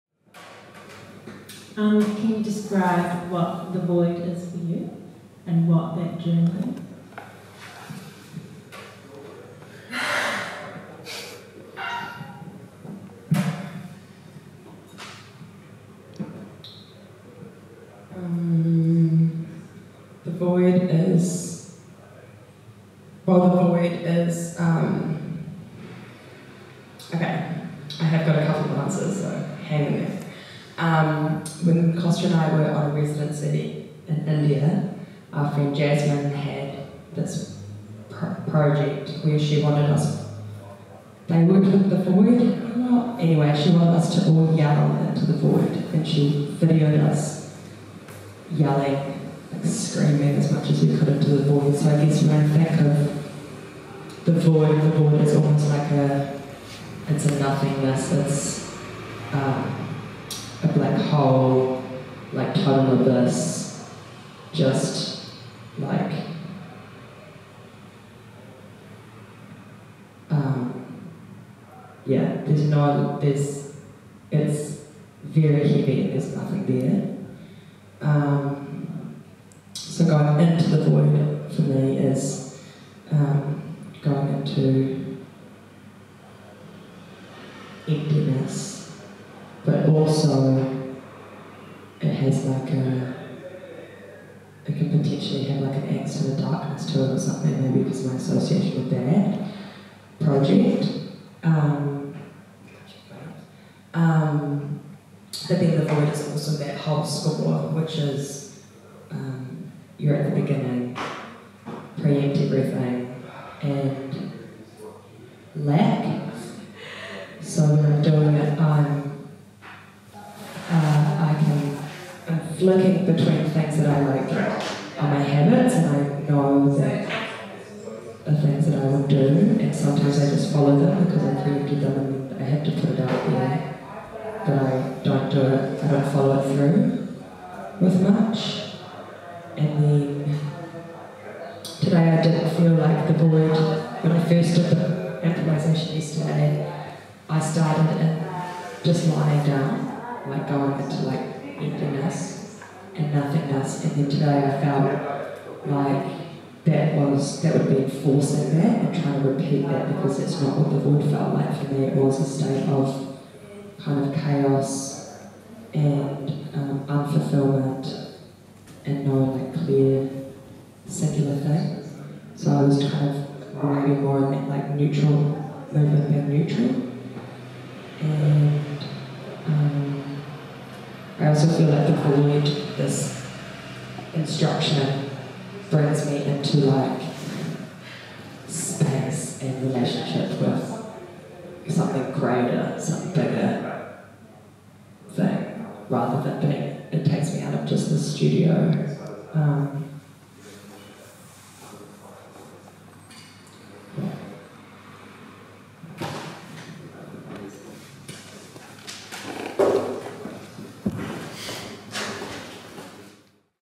Innate Series 2021 - Josie and Kosta by Fale
Exploring the openness of movement, the Innate series in 2021 are process-driven works to take us into new spaces of discovery, disruption and connectedness.

These exciting new residencies see the company collaborate with a diverse range of New Zealand choreographers to forge new synergies between artists, spaces, and communities. Each distinct work aims to provoke and discover—what is dance? How we do respond to the now?

Taking place throughout 2021, NZDC joined with Claire O’Neil, Joshua Faleatua, Katrina Bastian, Josie Archer and Kosta Bogoiveski for the first Innate works.

Josie Archer & Kosta Bogoievski
A process-driven contemporary dance occupation of Monster Valley shop front on Auckland’s infamous K Road, this work by Josie Archer and Kosta Bogoievski celebrates the ‘making’ and ‘doing’ as a product itself. The performance scores, text and brainstorms are all part of the work as dance and choreography develops.

Fale
Combining dance and filmmaking, Fale is working alongside us to uncover richness at the intersection of movement and digital storytelling, as our 2021 'sustained residency - digital artist'.

Film: Joshua Faleatua
Dance: Chrissy Kokiri, Kosta Bogoievski, Josie Elizabeth Archer, Amit Noy, Oliva McGregor